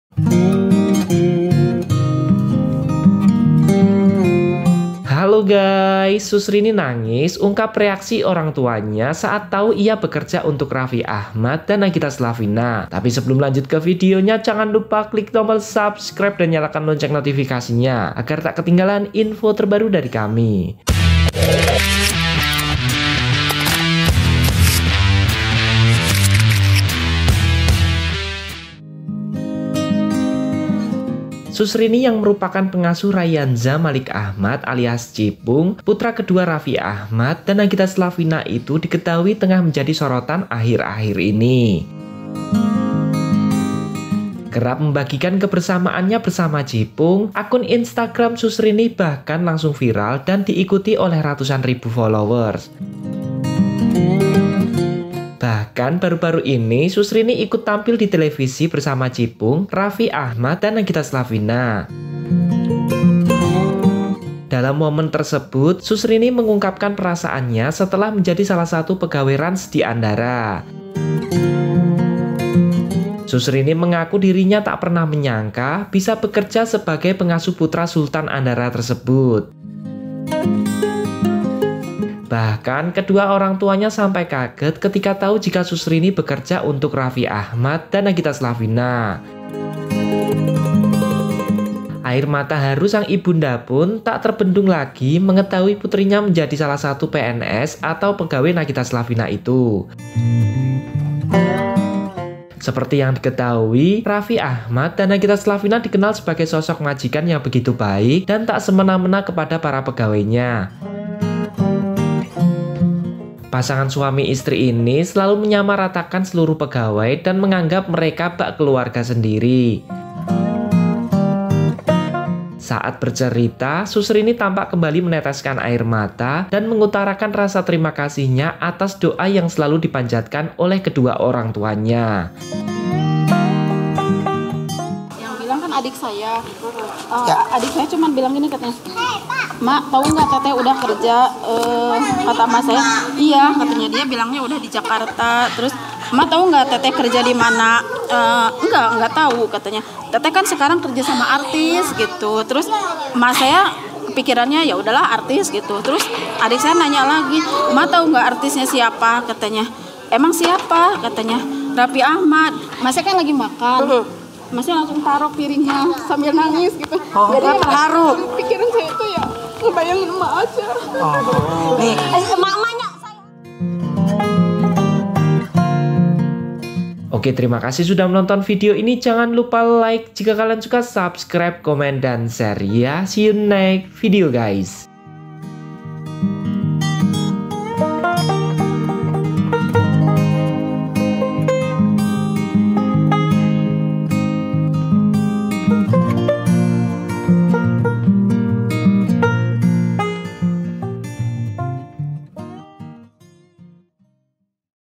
SUS RINI NANGIS, UNGKAP REAKSI ORANGTUAnya SAAT TAU IA BEKERJA UNTUK RAFFI AHMAD & NAGITA SLAVINA
Sus Rini yang merupakan pengasuh Rayyanza Malik Ahmad alias Cipung, putra kedua Raffi Ahmad dan Nagita Slavina itu diketahui tengah menjadi sorotan akhir akhir ini.

Kerap bagikan kebersamaannya bersama Cipung, akun instagram sus rini bahkan langsung viral dan diikuti oleh ratusan ribu follower.

Bahkan, baru baru ini Sus rini ikut tampil di televisi bersama cipung, Raffi ahmad dan nagita slavina.

Sala kesempatan tersebut, Sus Rini mengungkapkan perasaannya setelah menjadi salah satu pegawai RANS di Andara.

Sus Rini mengaku dirinya tak pernah menyangka bisa bekerja sebagai pengasuh dari putra Sultan Andara itu.

Bahkan, kedua orangtuanya sampai kaget ketika tau jika sus rini bekerja untuk Raffi ahmad dan Nagita slavina.

Air mata haru sang ibunda pun tak terbendung lagi mengetahui putrinya menjadi salah satu PNS atau pegawai Nagita slavina itu.

Seperti yang diketahui, Raffi ahmad dan Nagita slavina dikenal sebagai sosok majikan yang begitu baik dan tak semena mena kepada para pegawainya..

Pasangan suami istri ini selalu menyamaratakan seluruh pegawai dan menganggap mereka bak keluarga sendiri.

Saat bercerita, Sus Rini tampak kembali meneteskan air mata dan mengutarakan rasa terima kasihnya atas doa yang selalu dipanjatkan oleh kedua orang tua.